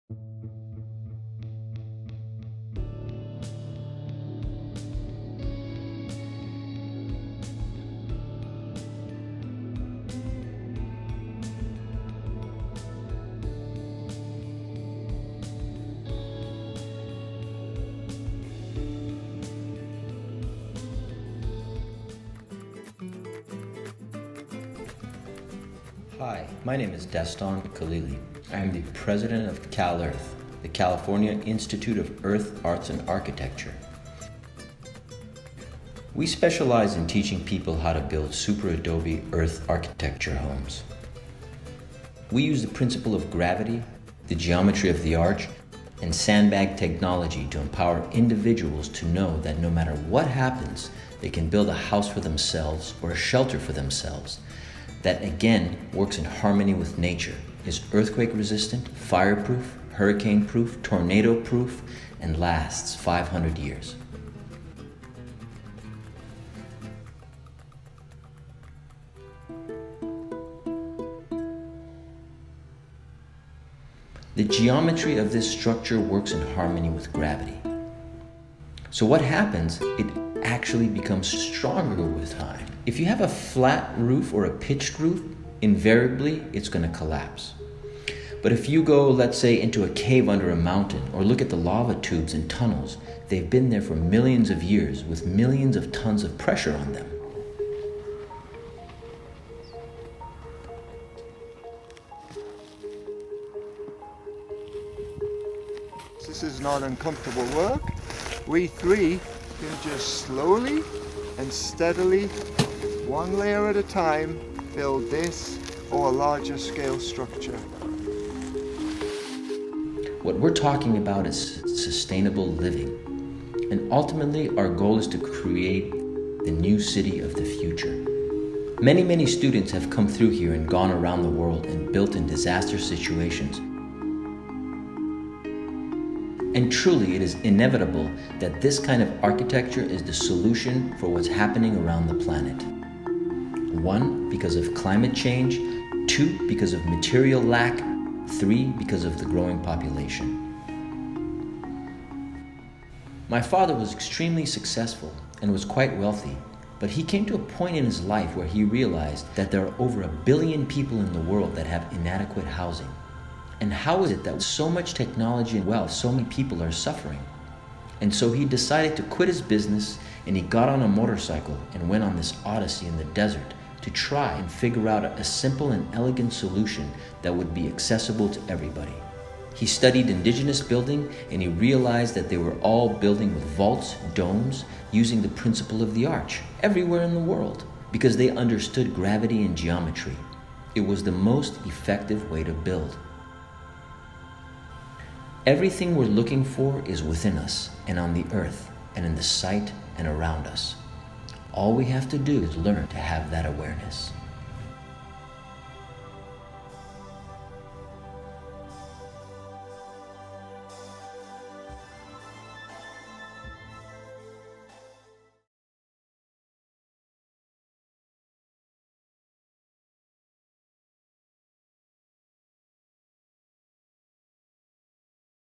The strange homes built for Mars - BBC REEL
Could these homes, once designed for life on Mars, solve the housing crisis?